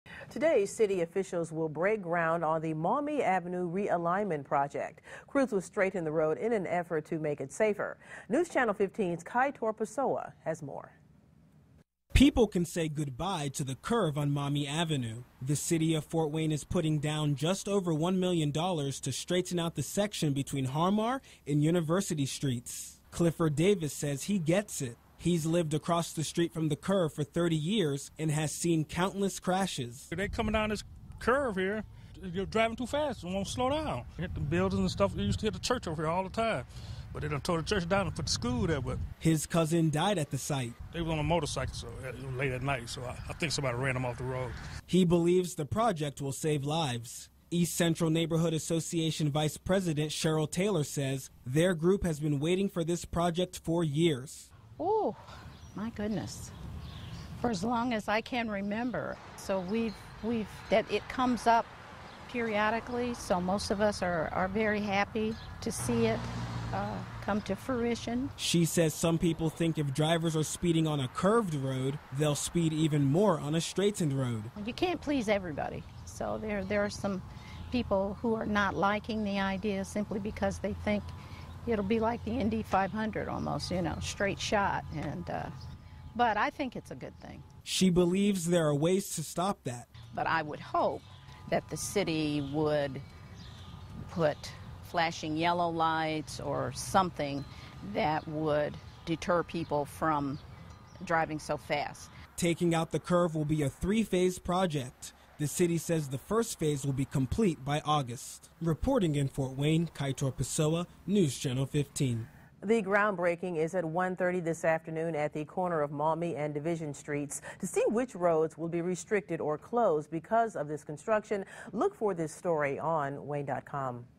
Maumee Ave. Getting Straightend
After causing numerous vehicle crashes, the curve in Maumee Ave. is getting straightened out.